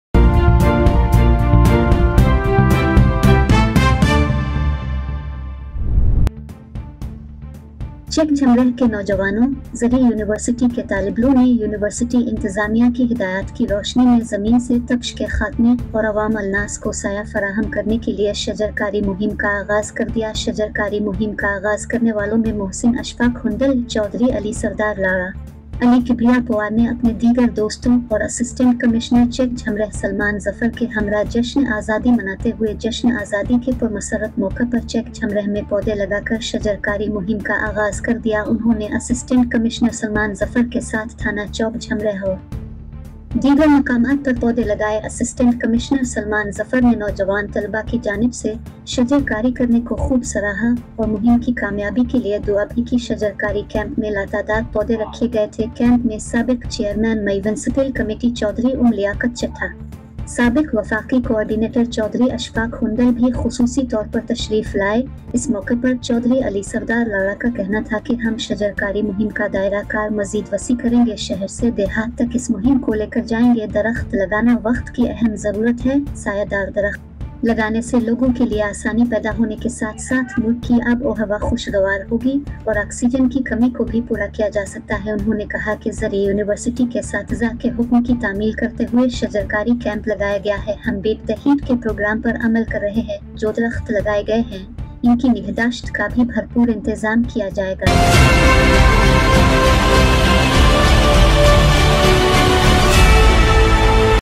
Initiation of tree planting campaign to eliminate heat from land and provide shade to the people
Pakistan Communicate World Network#pcworldnewshd